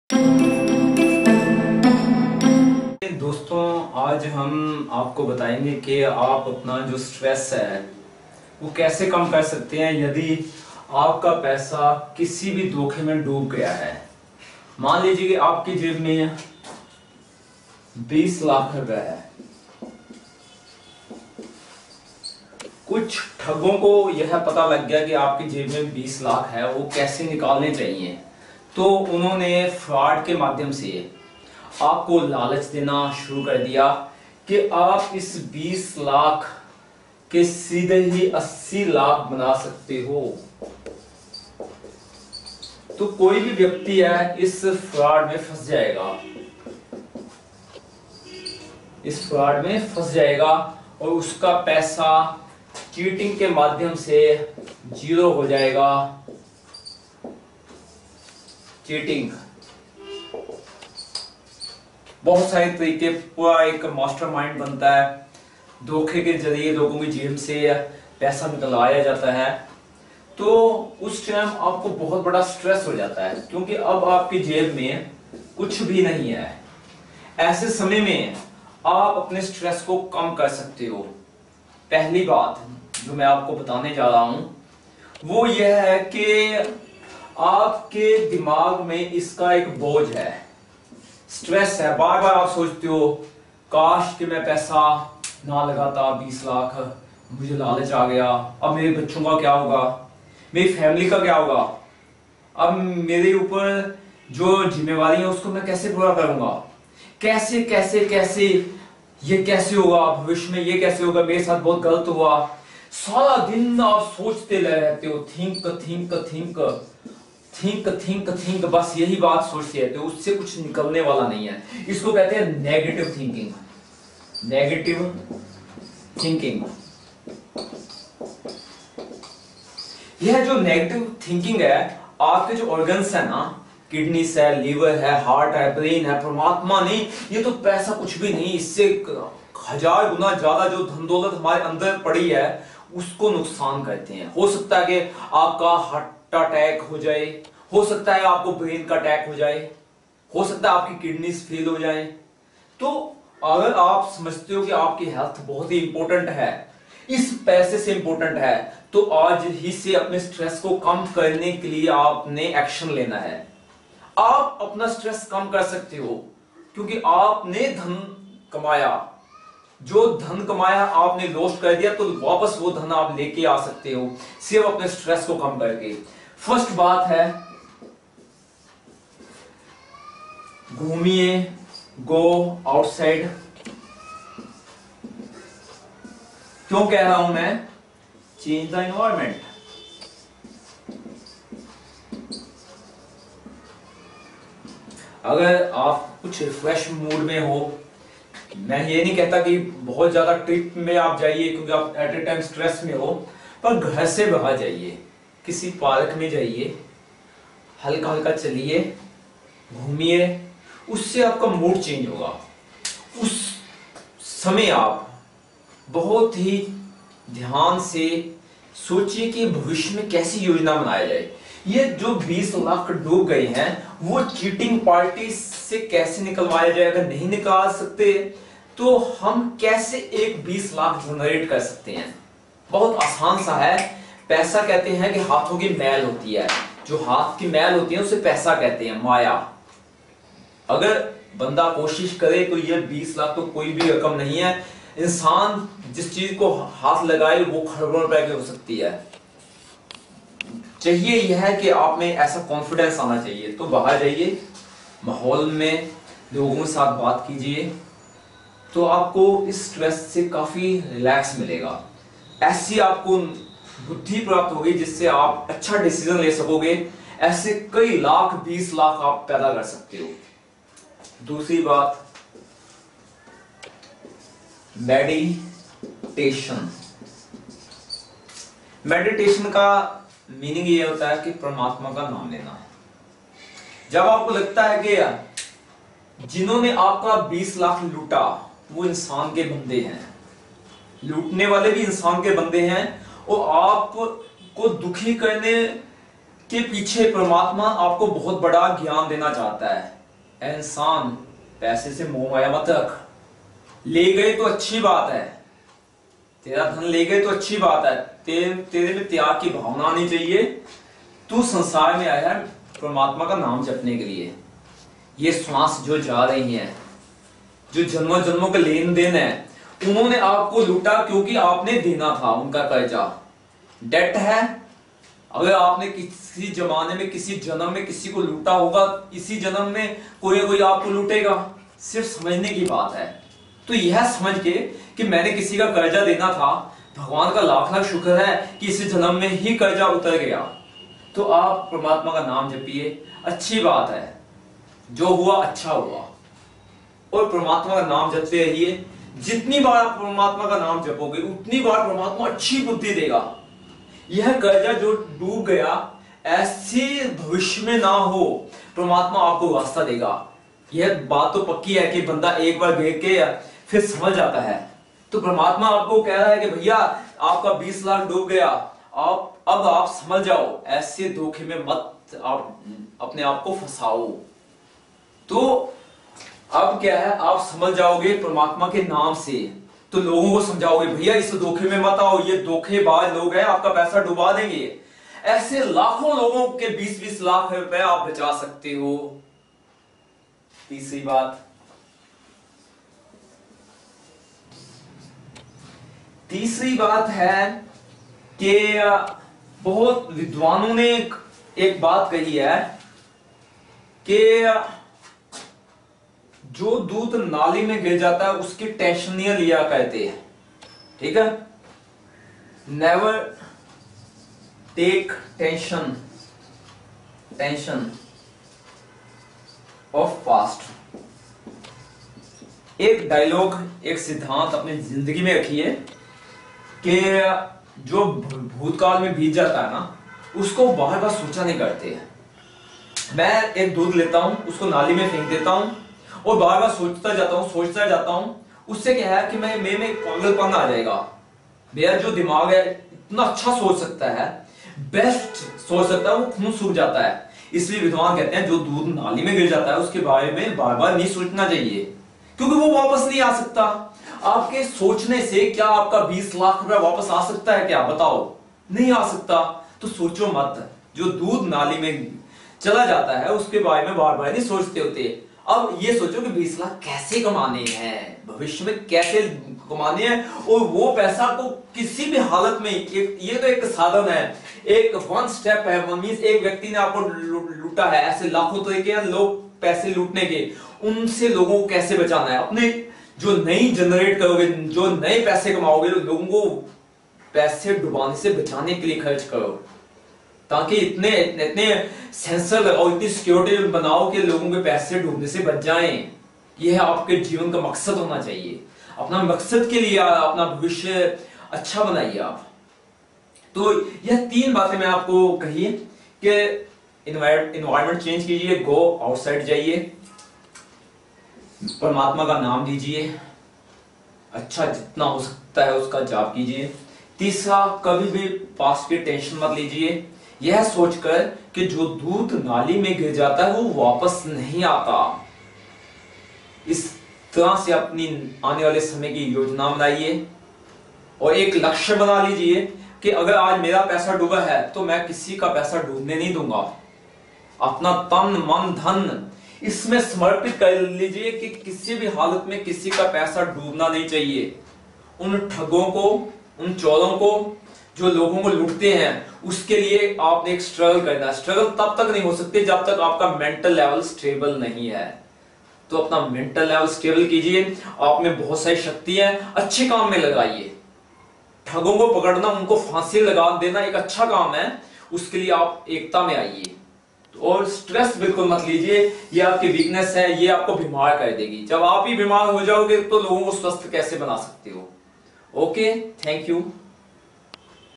How to Remove Stress of Lost Money in Fraud
If you have lost your big and hard earned money in fraud, it is naturally, you will go to stress. This stress is very danger to your health. So, for removing stress, you should follow my three basic rules.